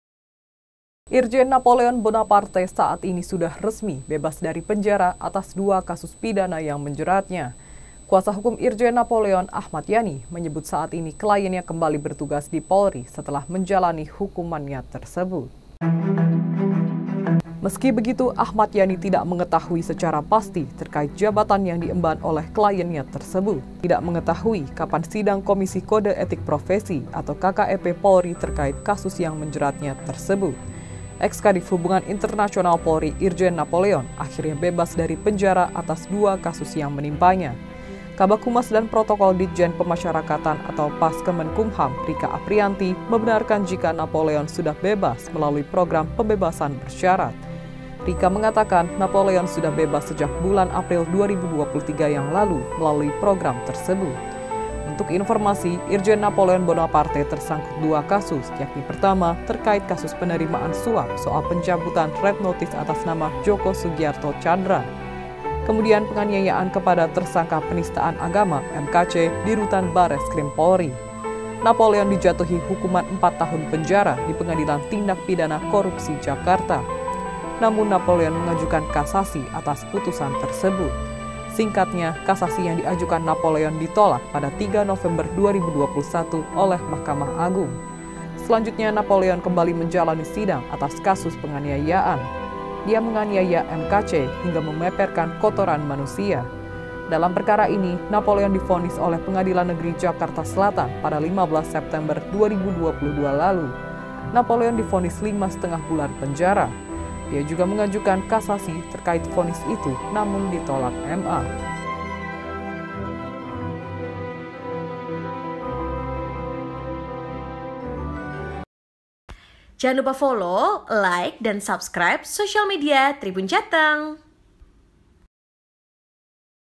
Viral Irjen Napoleon Bebas Penjara Kembali jadi Polisi
Irjen  Napoleon Bonaparte saat ini sudah resmi  bebas dari penjara atas dua kasus pidana yang menjeratnya.

Kuasa hukum Irjen Napoleon, Ahmad Yani menyebut saat ini kliennya kembali bertugas di  Polri setelah menjalani hukumannya tersebut.

Meski begitu, Ahmad Yani tidak mengetahui secara pasti terkait jabatan yang diemban oleh kliennya tersebut.

Di sisi lain, Ahmad Yani juga tidak mengetahui kapan sidang Komisi Kode Etik Profesi (KKEP)  Polri terkait kasus yang menjeratnya tersebut.

Eks Kadiv Hubungan Internasional  Polri Irjen Napoleon akhirnya  bebas dari penjara atas dua kasus yang menimpanya.

Host : Elsa Elningtyas
Editor : Yga